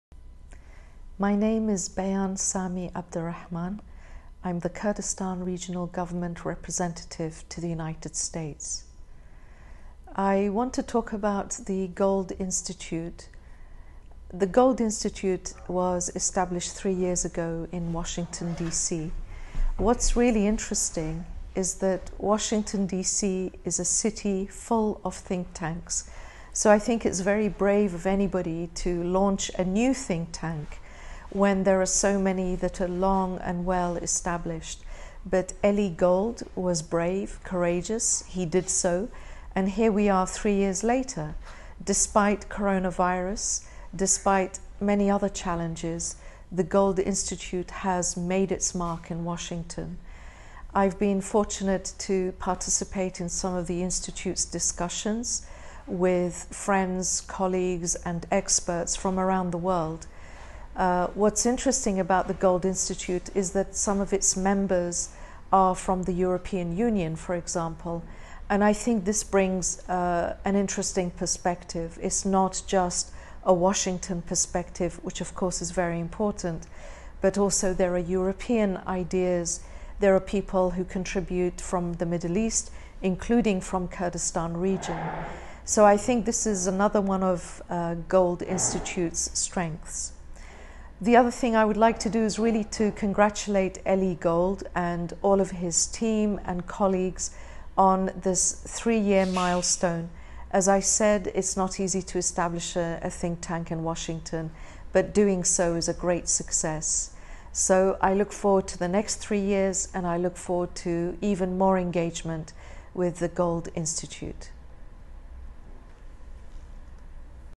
A Message from Hon. Bayan Sami Rahman Kurdistan Regional Gov Rep. to USA to the Gold Institute
A Message from Hon. Bayan Sami Abdul Rahman the Kurdistan Regional Gov Representative to USA on the occasion of the Gold Institute for International Strategy's 3rd Anniversary